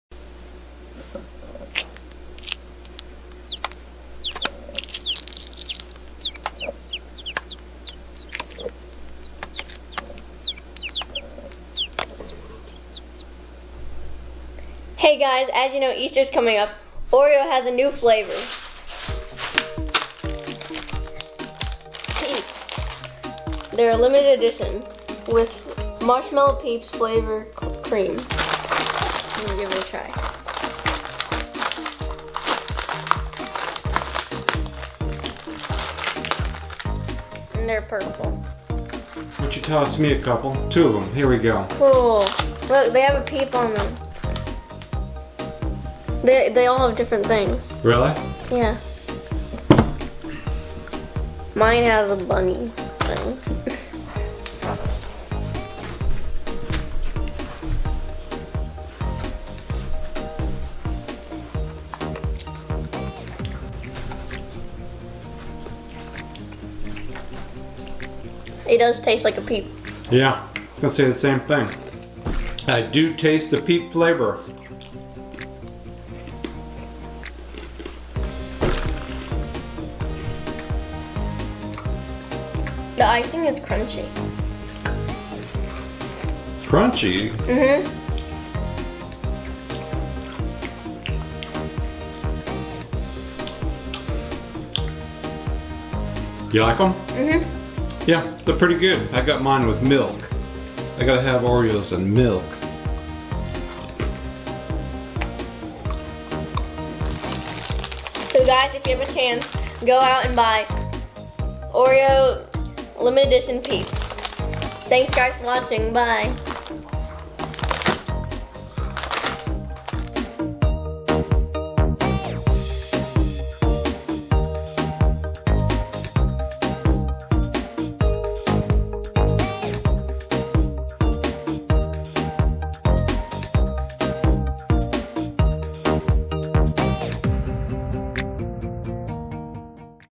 Peeps Oreos (Limited Edition for Easter)
Peeps Oreos. Today we give the new Oreo Peeps a try!